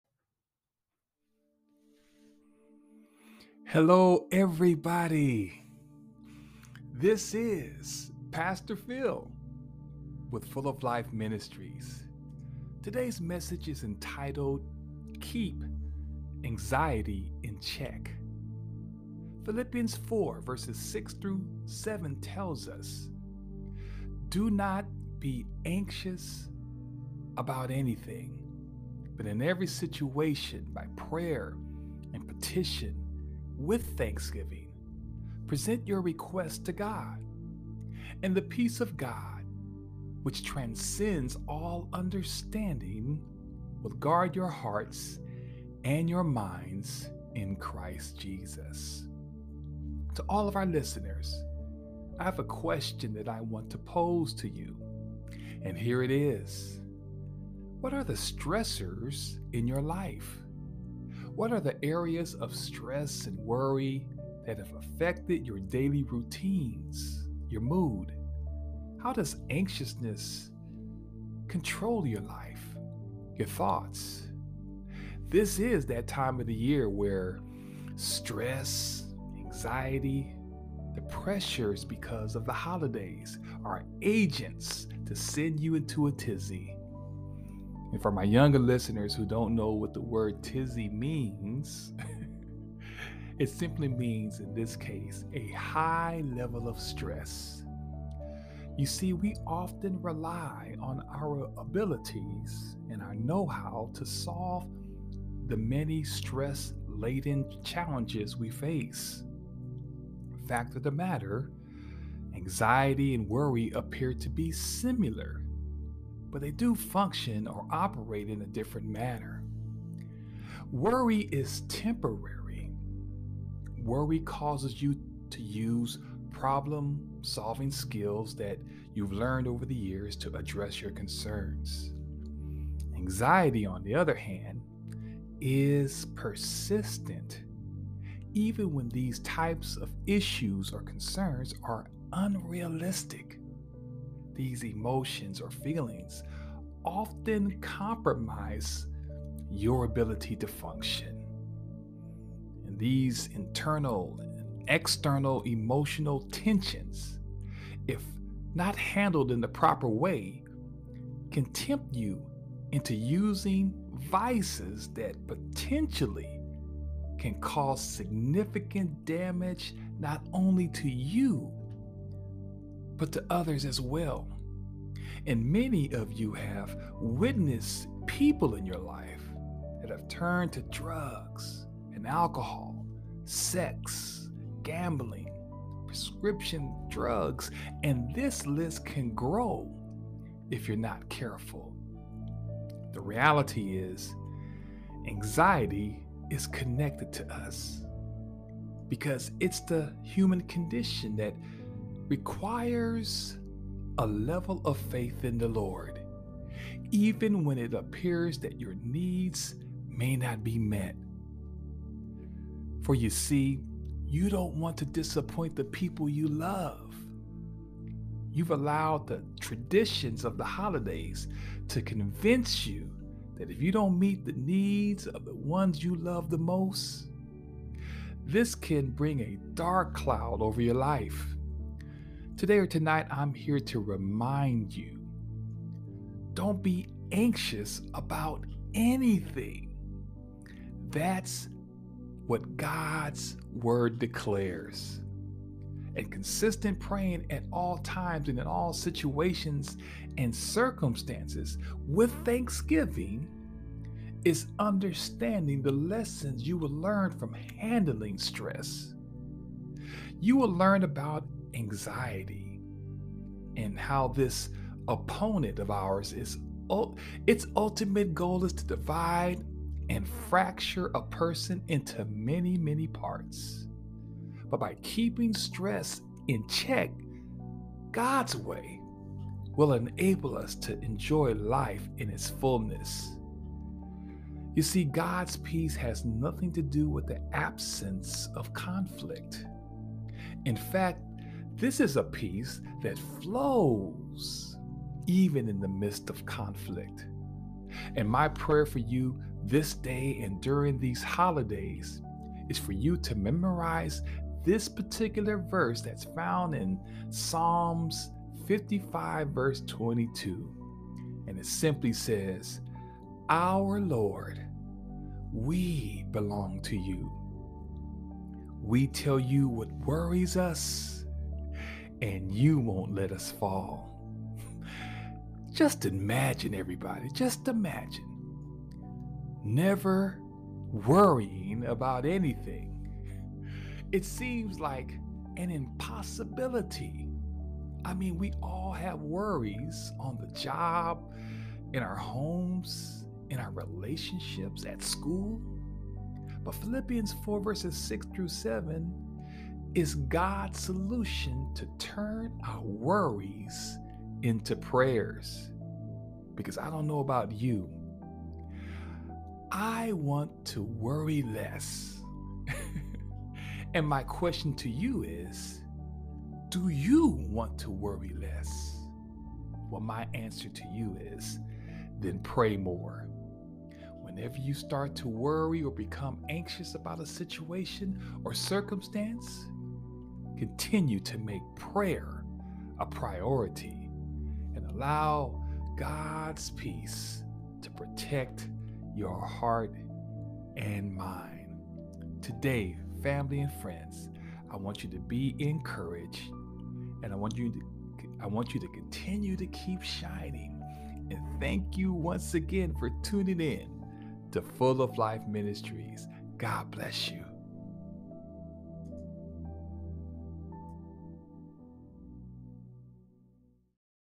"Keep Anxiety in Check!"
What are the stressors in your life? What are the areas of stress and worry that have affected your daily routines, your mood? It's time to keep anxiety in check!